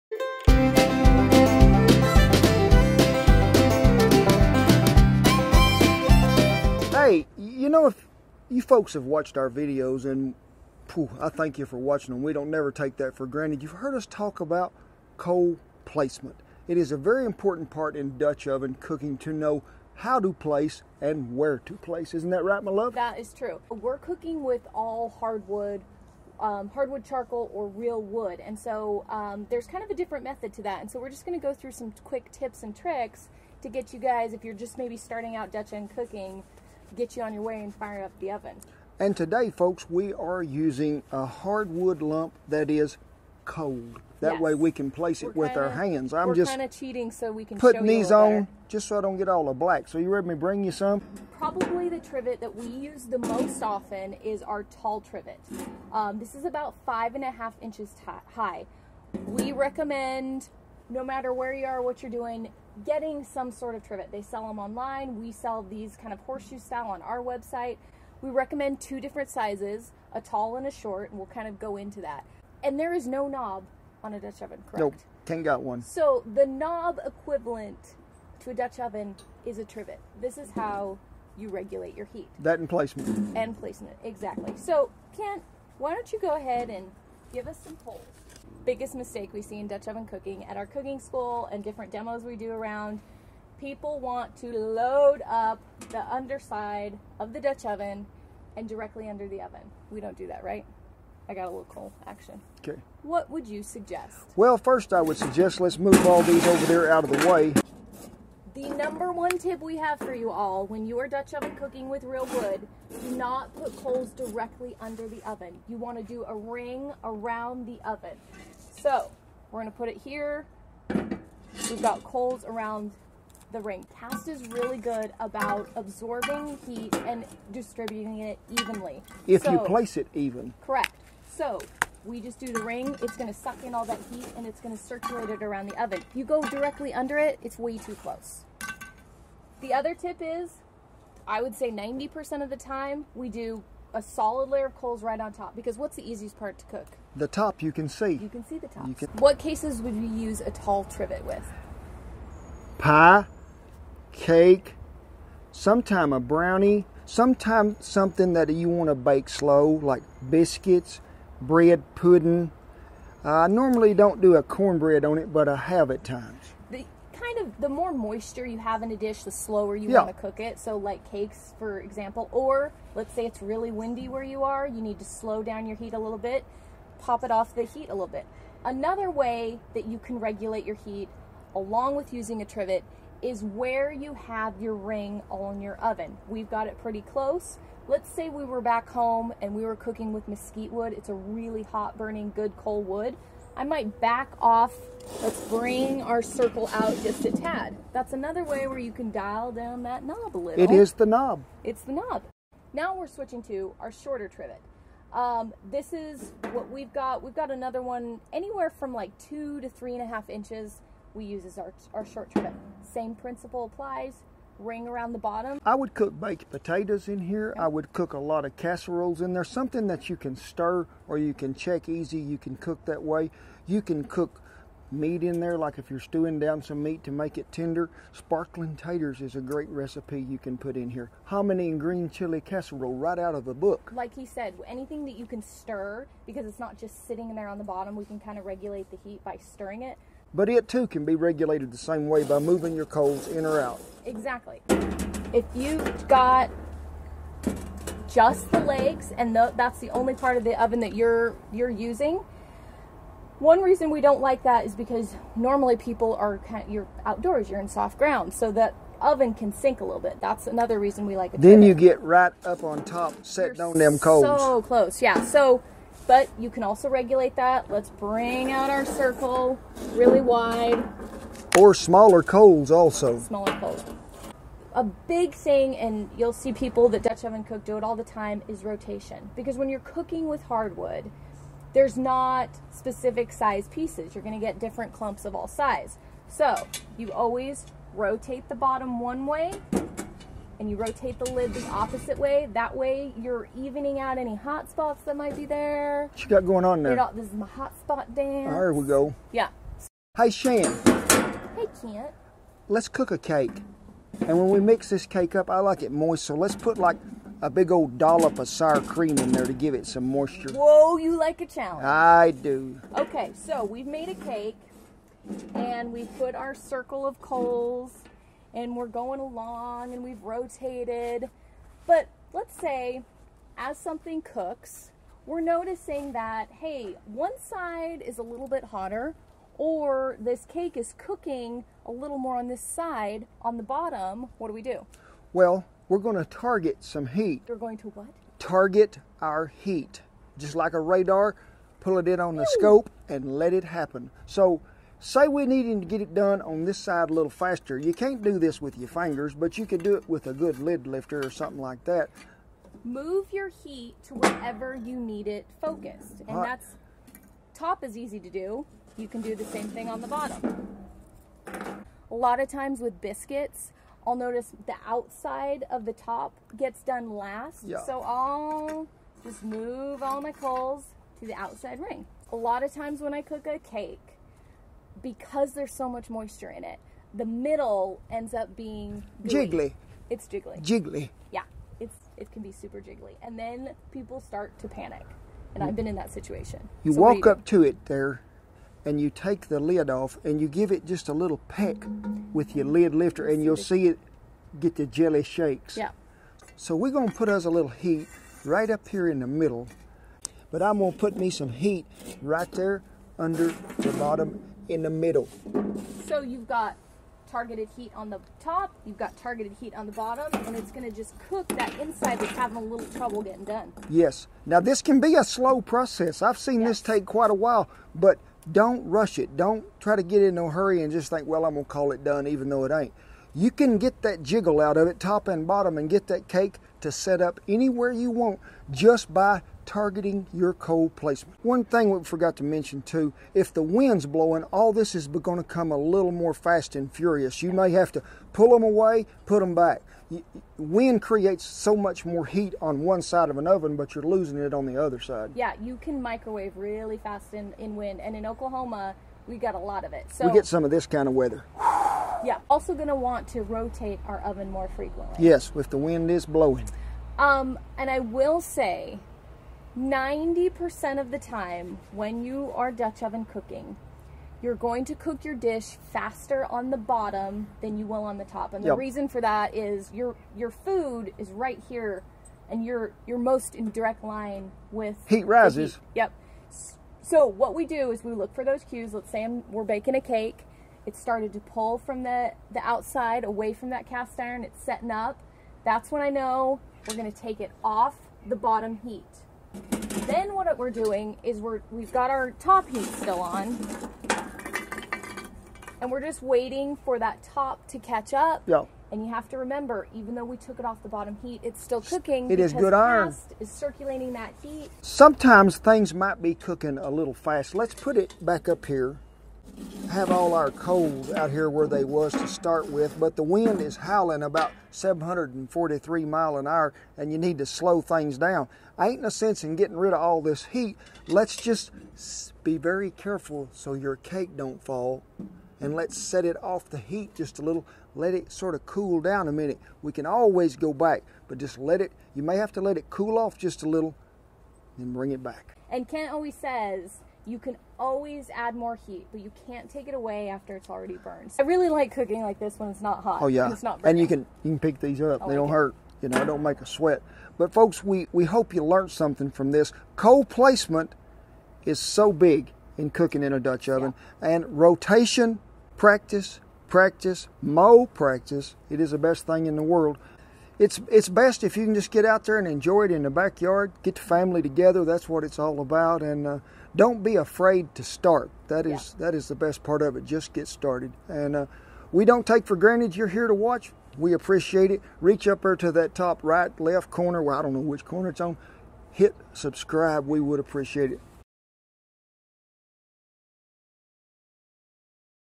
Basic Coal Placement for Dutch Oven Cooking with Hardwood
In this video we're giving you some basic and beginner tips on how to place hardwood coals when Dutch oven cooking. We like using real wood or hardwood lump charcoal because of it's great heat it's also what we use when cooking on ranches, etc. With just a little practice you'll get the hang of cooking with hardwood with Dutch ovens!

Kent Rollins
Chuck Wagon Cook, Grilling, Dutch Oven Cooking, Cowboy, Cast Iron